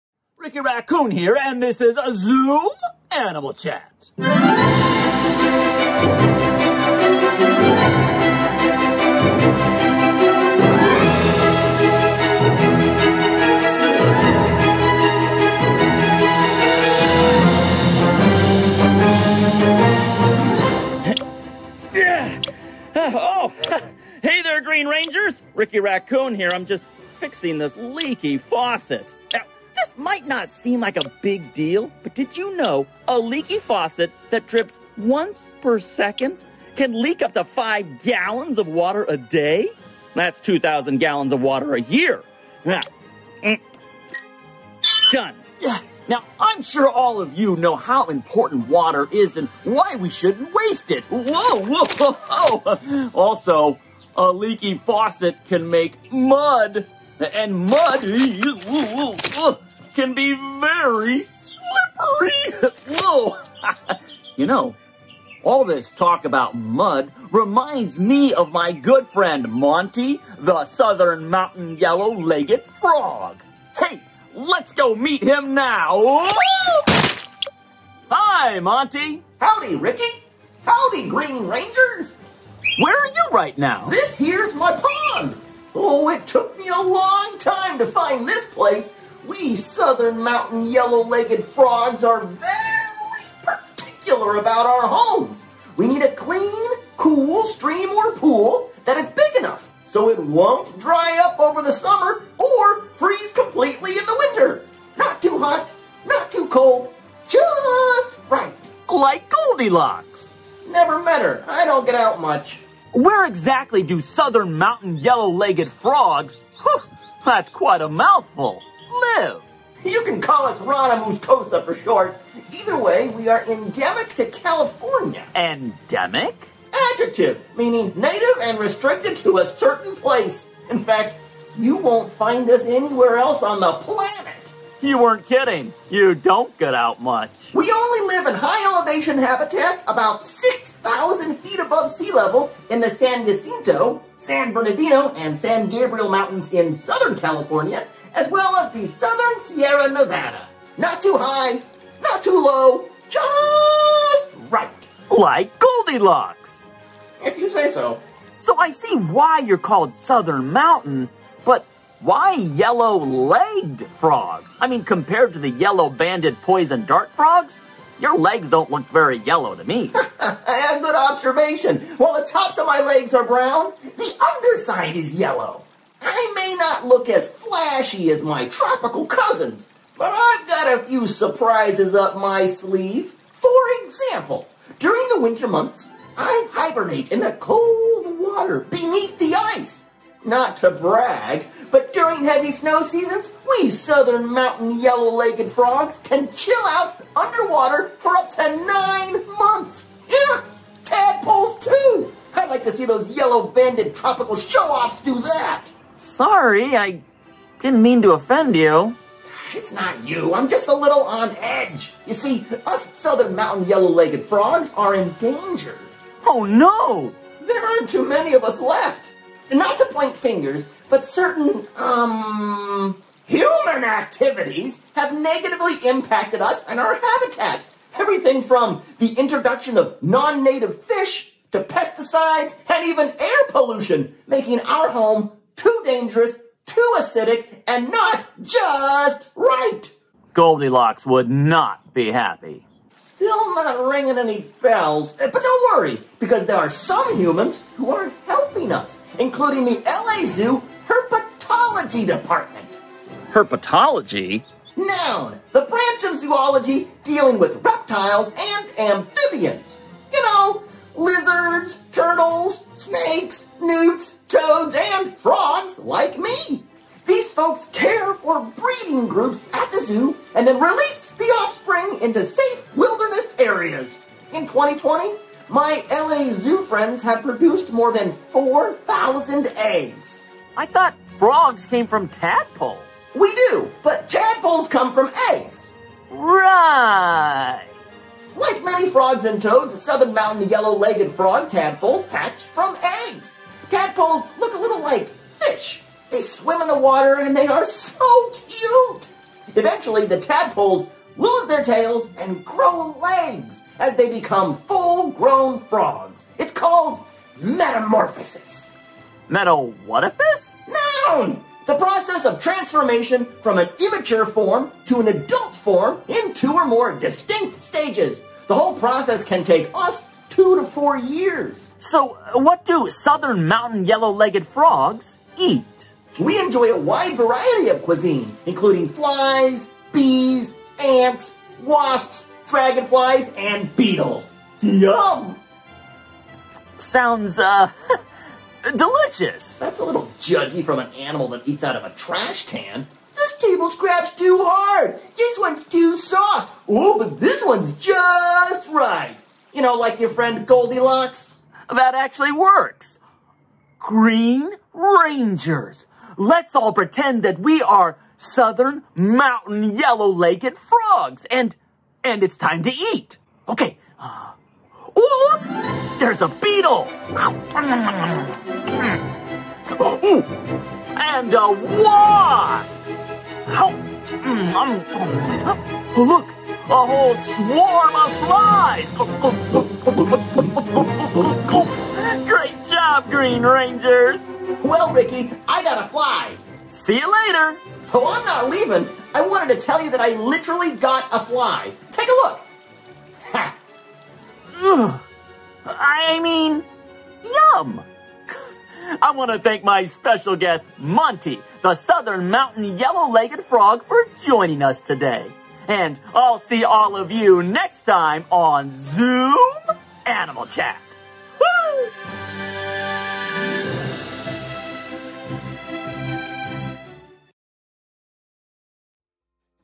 L.A. ZOO-m Animal Chat with Swazzle ft Monty the Southern Mountain Yellow-Legged Frog
The Los Angeles Zoo is very hoppy to present an "L.A. ZOO-m Animal Chat" featuring one of our favorite frog species! Jump into some fascinating herpetological facts as environmental superhero Ricky the Racoon interviews Monty the Southern Mountain Yellow-Legged Frog. It's the third episode in our series of chats produced by @SwazzleProductions as part of our extended celebration of wildlife conservation and sustainability, Wild for the Planet.